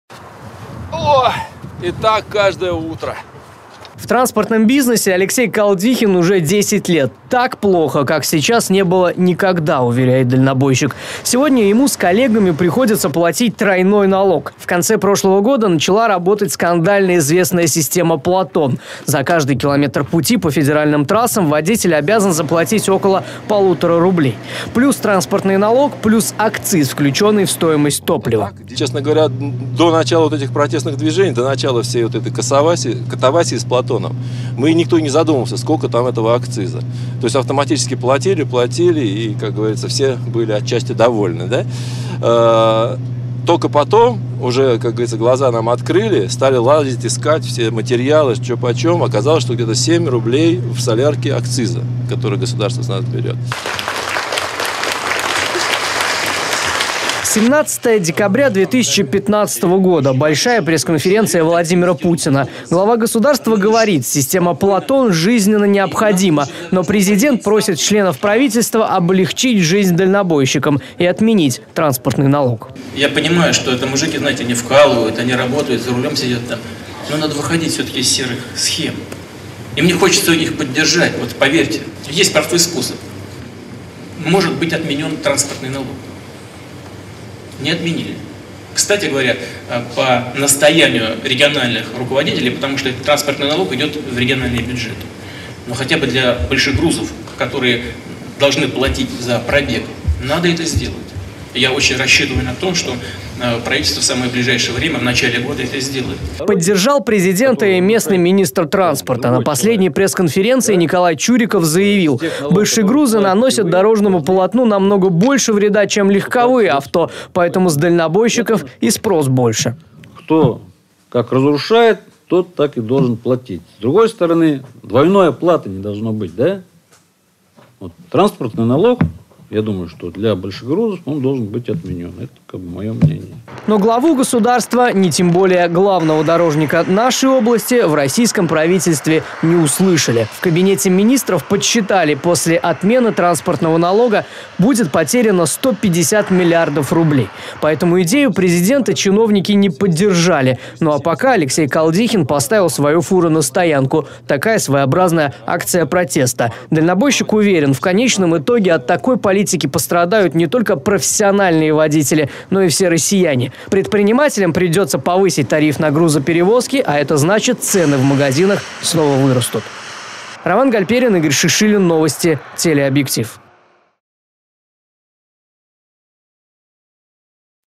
Транспортный налог на грузовые автомобили решили оставить
Не указ! Правительство России оставляет транспортный налог на грузовые автомобили. С предложением отменить сбор выступил президент России Владимир Путин. После изучения вопроса члены кабинета министров подсчитали, потери от такого шага достигнут 150 миллиардов рублей. А отказываться от таких денег чиновники не хотят...